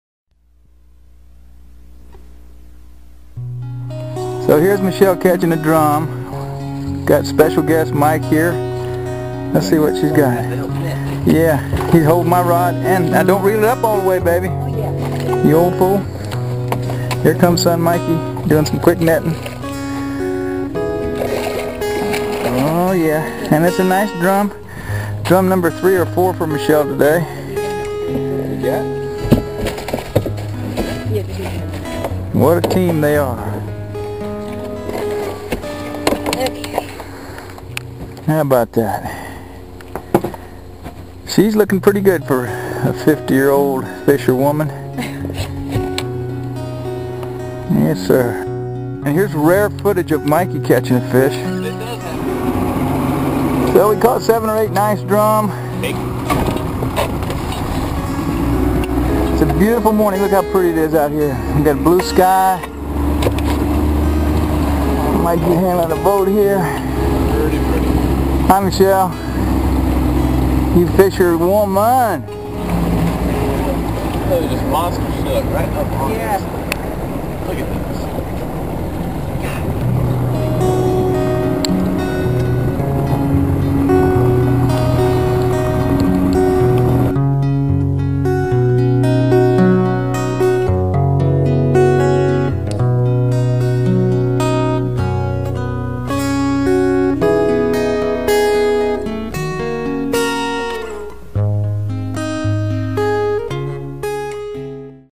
Fishing with Mike and Michele
Mikey and Mish catching drum